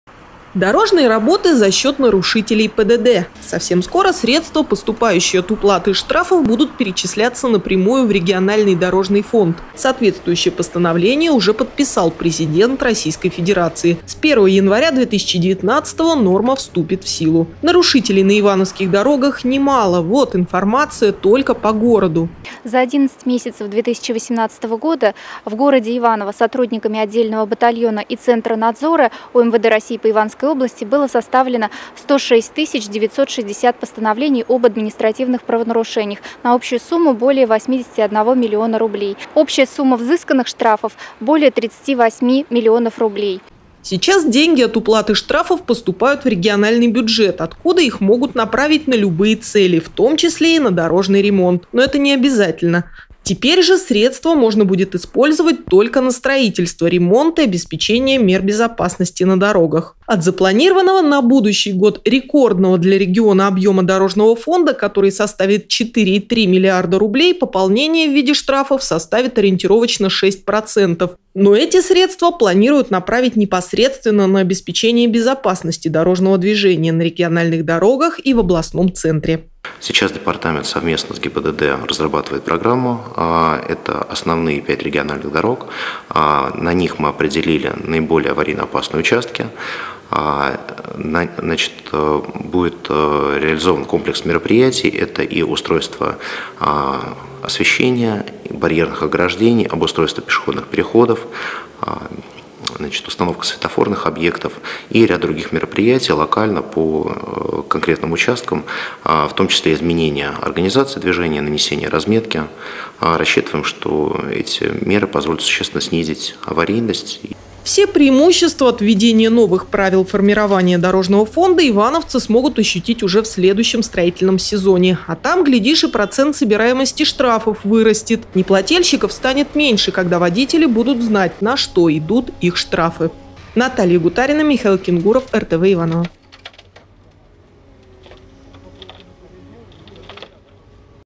Дорожный ремонт за счет нарушителей
Совсем скоро средства, поступающие от уплаты штрафов, будут перечисляться напрямую в региональный дорожный фонд. Соответствующее постановление уже подписал президент Российской Федерации, с 1 января 2019 норма вступит в силу.